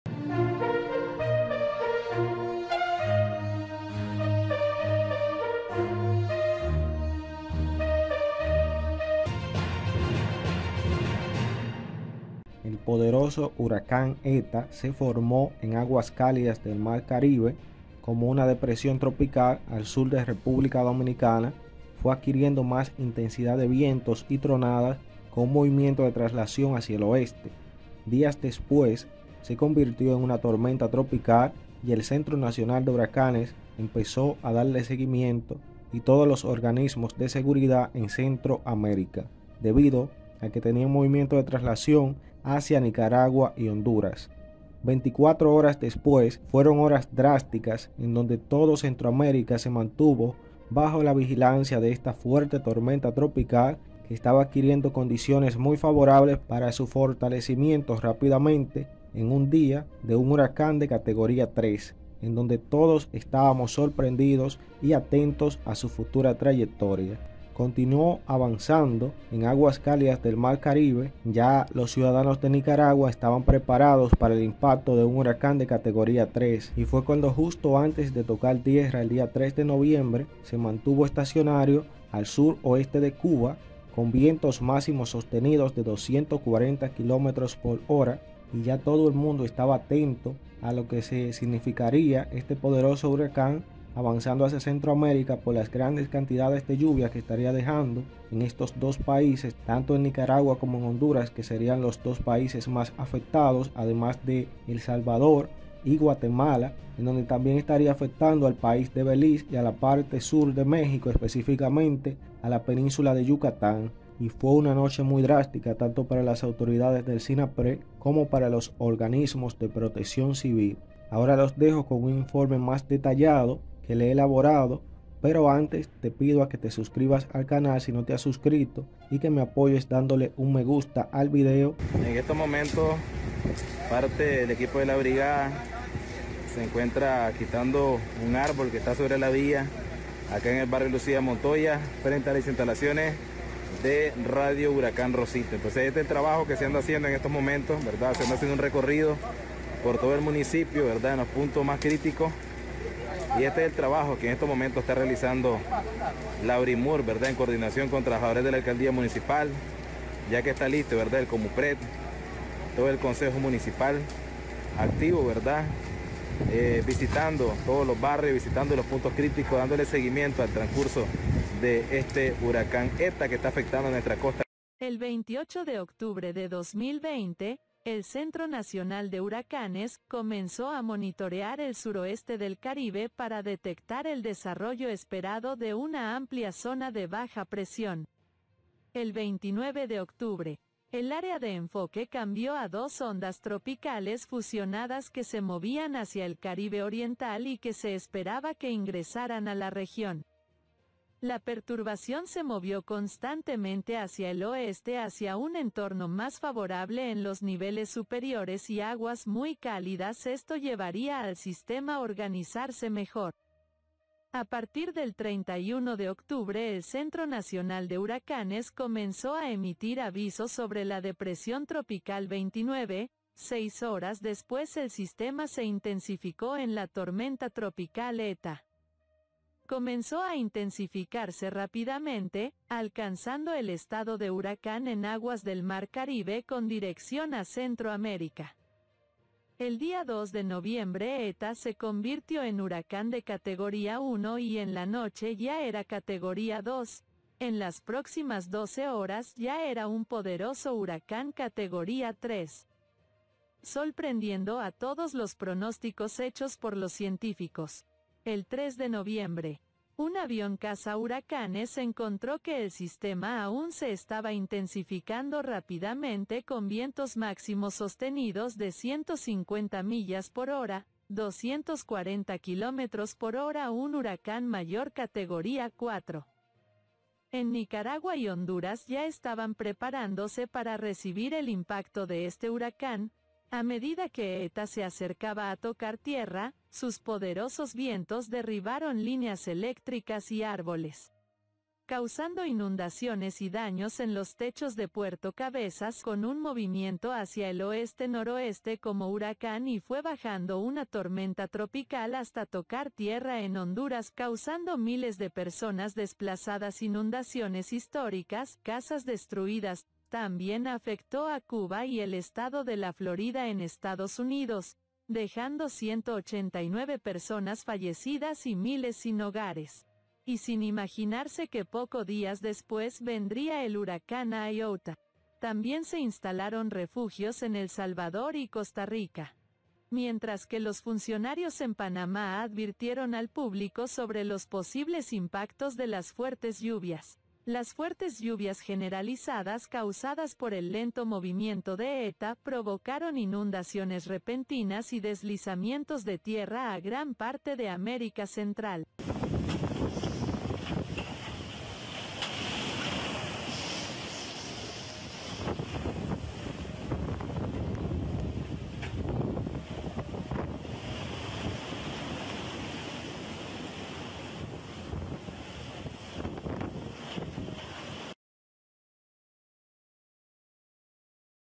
UN AÑO DEL PODEROSO HURACÁN ETA, CENTRO AMÉRICA, HONDURAS, NICARAGUA, El Caribe
El ojo del huracán Eta, con vientos máximos de 240 kilómetros por hora, tocó tierra este martes al sur del municipio de Bilwi o Puerto Cabezas, en el Caribe norte de Nicaragua, informaron las autoridades nicaragüenses El Centro Nacional de Huracanes empezó a emitir avisos de Tormenta Tropical al sur de República Dominicana, 12 Horas después eta se convirtió en Huracán categoría 1 y ese mismo día ya era un Poderoso Huracán categoría 3, afectó en su trayectoria con vientos fuertes y lluvias a Nicaragua, Honduras, Guatemala, Belice, México, Cuba y Estados Unidos

El 28 de octubre de 2020, el Centro Nacional de Huracanes, comenzó a monitorear el suroeste del Caribe para detectar el desarrollo esperado de una amplia zona de baja presión. El 29 de octubre, el área de enfoque cambió a dos ondas tropicales fusionadas que se movían hacia el Caribe Oriental y que se esperaba que ingresaran a la región, La perturbación se movió constantemente hacia el oeste hacia un entorno más favorable en los niveles superiores y aguas muy cálidas esto llevaría al sistema organizarse mejor

 a partir del 31 de octubre El Centro Nacional de Huracanes comenzó a emitir avisos sobre la depresión tropical veintinueve, Seis horas después el sistema se intensificó en la tormenta tropical Eta, comenzó a intensificarse rápidamente, alcanzando el estado de huracán en aguas del Mar Caribe con dirección a centro américa, el día 2 de noviembre eta se convirtió en Huracán de categoría 1 y en la noche ya era categoría 2, en las próximas 12 horas ya era un poderoso huracán categoría 3, sorprendiendo a todos los pronósticos hechos por los científicos

el 3 de noviembre, un avión casa huracanes encontró que el sistema aún se estaba intensificando rápidamente con vientos máximos sostenidos de 150 mph, 240 kilómetros por hora un Huracán mayor categoría 4

En Nicaragua y Honduras ya estaban preparándose para recibir el impacto de este huracán, pero no se imaginaban lo que venia a estos Países que seria algo tan fuerte, A medida que Eta se acercaba a tocar tierra, sus poderosos vientos derribaron líneas eléctricas y árboles, causando inundaciones y daños en los techos de Puerto Cabezas avanzo con un movimiento hacia el oeste noroeste como huracán y fue bajando una tormenta tropical hasta tocar tierra en Honduras por su centro. causando miles de personas desplazadas inundaciones históricas, casas destruidas también afectó a Cuba y el Estado de la Florida en Estados Unidos, dejando 189 personas fallecidas y miles sin hogares, y sin imaginarse que poco días después vendría el huracán iota

También se instalaron refugios en El Salvador y Costa Rica, mientras que los funcionarios en Panamá advirtieron al público sobre los posibles impactos de las fuertes lluvias. Las fuertes lluvias generalizadas causadas por el lento movimiento de Eta provocaron inundaciones repentinas y deslizamientos de tierra a gran parte de América Central